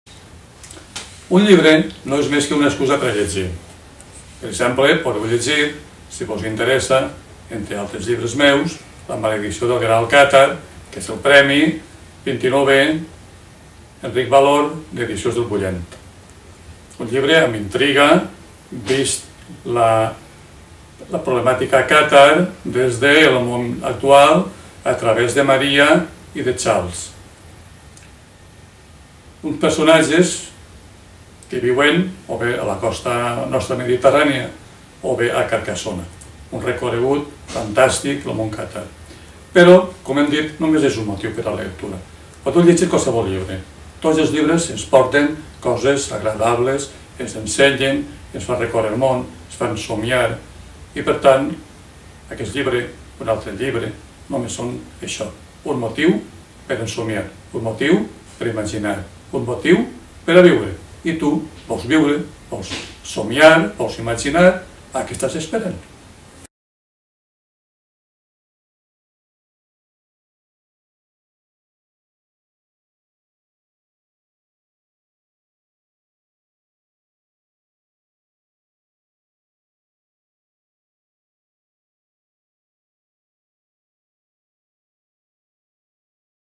Joan Pla La maledicció del graal càtar
L'escriptor Joan Pla ens parla de la lectura i del seu llibre "La maledicció del graal càtar", que va guanyar el 29é Premi Enric Valor.

Maria no se sent còmoda a casa. Hi ha els típics conflictes familiars amb els pares i un germà drogoaddicte. En vista de la por que pren al seu germà, fuig a Carcassona on viu Charles. Però amb ella s'enduu una estranya medalla que conté una maragda. Només quan la llueix en un dels actes culturals que es fan a l'estiu a Carcassona, Monsieur Brouart la reconeixerà i començarà una llarga i trepidant història per aconseguir el graal càtar, sobre el qual pesa una greu maledicció que s'anirà complint sense excepcions.